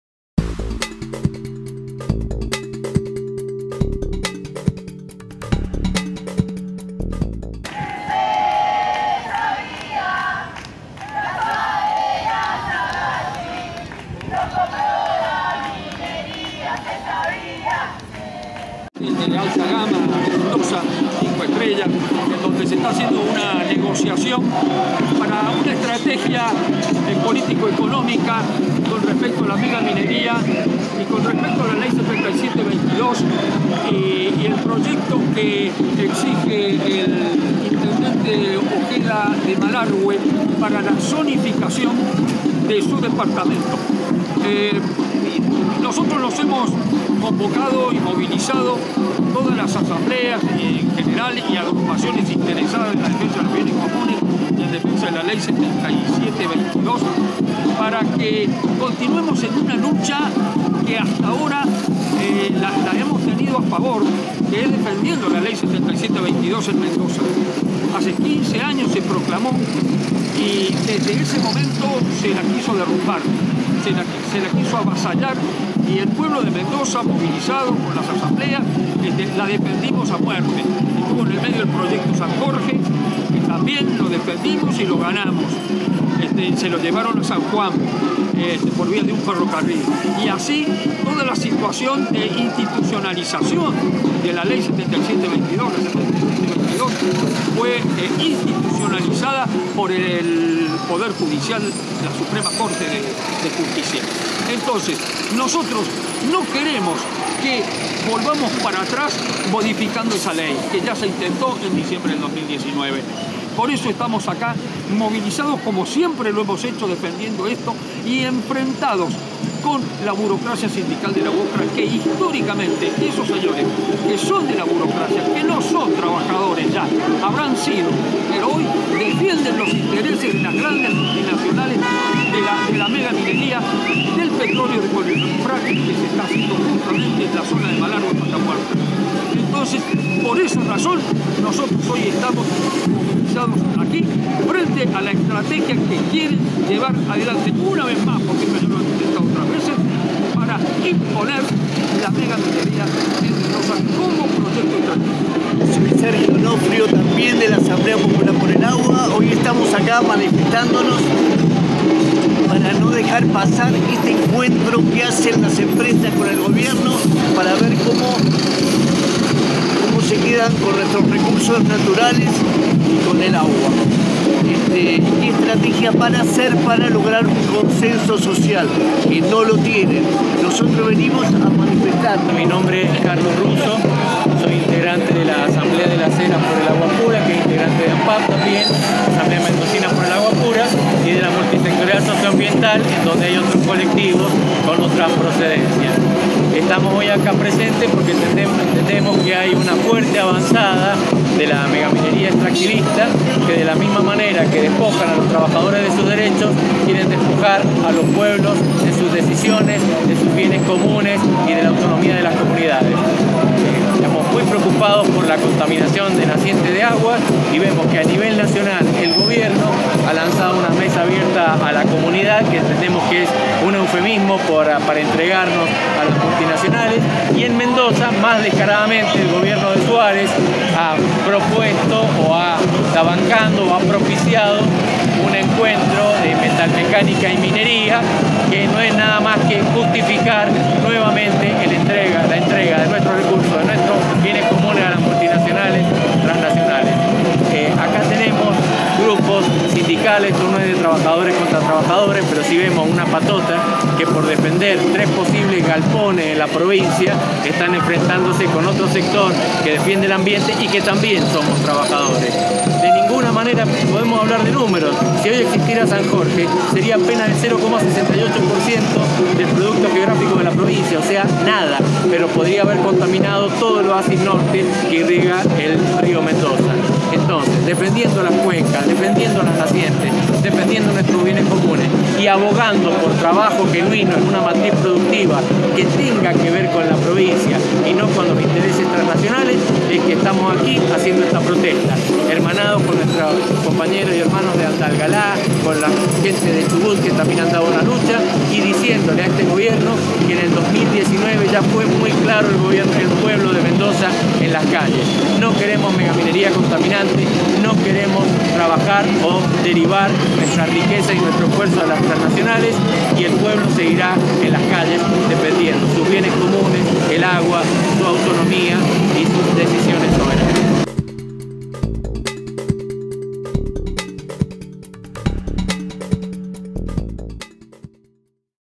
Asambleas y organizaciones socioambientales se manifestaron contra el lobby minero en Mendoza
El martes 18 de mayo desde las 8.30hs asambleas populares de los distintos departamentos de Mendoza y organizaciones sociales se manifestaron frente al Hotel Hyatt, donde se realizó el Foro Minero. El banderazo rechazó lo allí expuesto por empresarios y funcionarios públicos respecto de los beneficios de la explotación minera en la Cordillera de Los Andes y llamó a estar en alerta en defensa de los bienes comunes en todas las provincias vecinas. Las consignas La7722NoSeToca ElAguaDeMendozaNoSeNegocia siguen presentes y fortalece la lucha de las asambleas y la ciudadanía en defensa de nuestro bien más preciado, el agua.